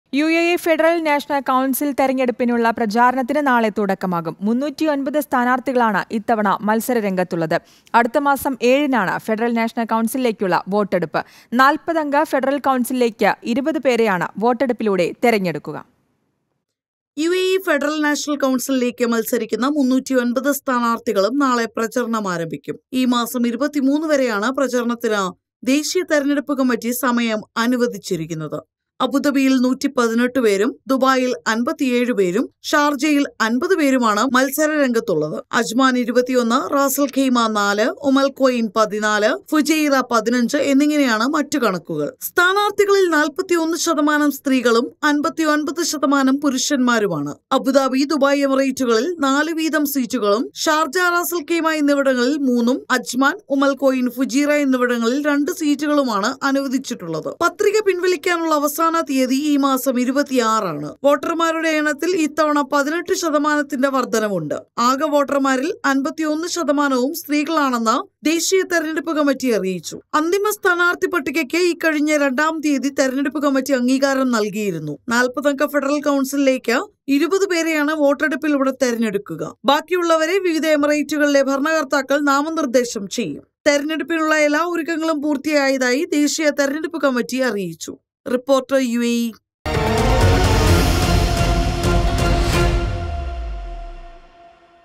യുഎഇ ഫെഡറൽ നാഷണൽ കൗൺസിൽ തിരഞ്ഞെടുപ്പ് പ്രചാരണത്തിന് നാളെ തുടക്കം | UAE
With Regards
Team RBC